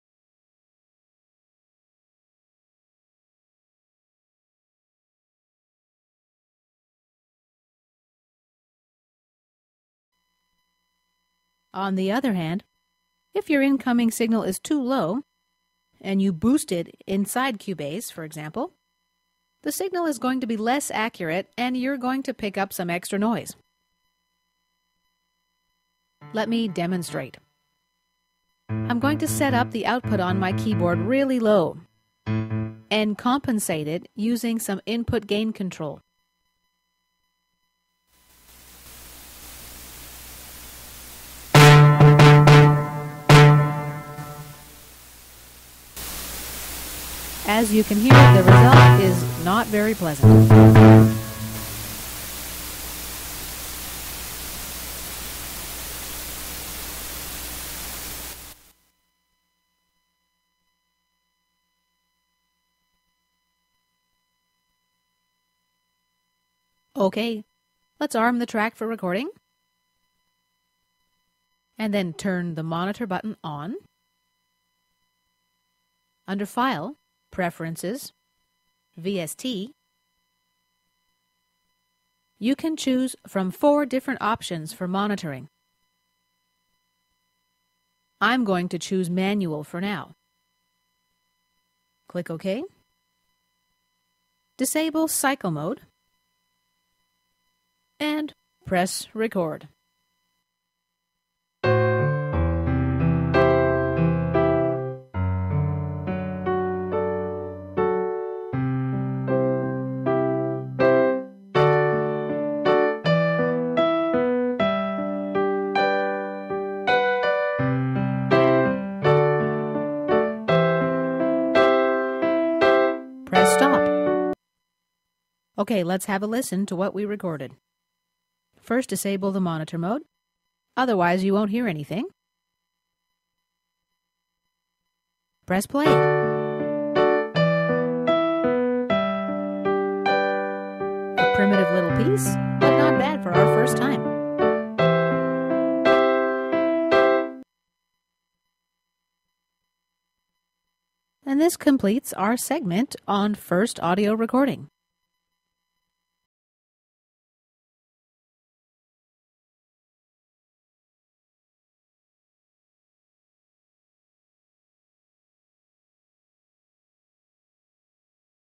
09 Cubase Video Tutorial - First Audio Recording - part 2 of 2
- In this Cubase video tutorial, I take you through your first audio recording. This is part 2 of 2.